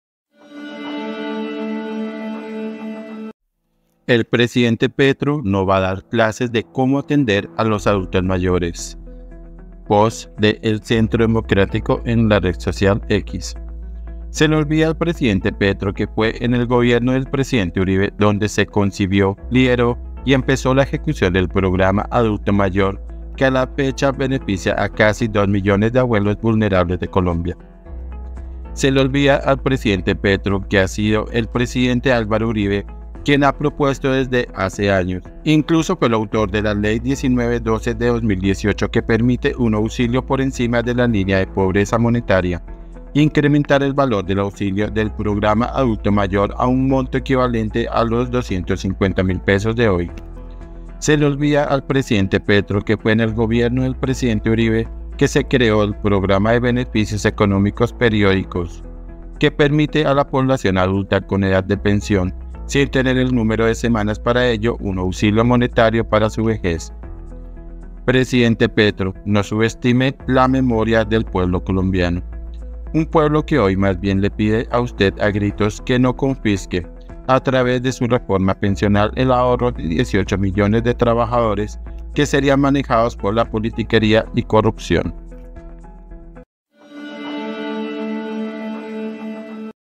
"SE LE OLVIDA AL PRESIDENTE PETRO..." 📢 Duro Mensaje
Se le olvida al presidente Petro que fue en el gobierno del presidente Uribe donde se concibió, lideró y empezó la ejecución del programa Adulto Mayor que a la fecha beneficia a casi 2 millones de abuelos vulnerables de Colombia.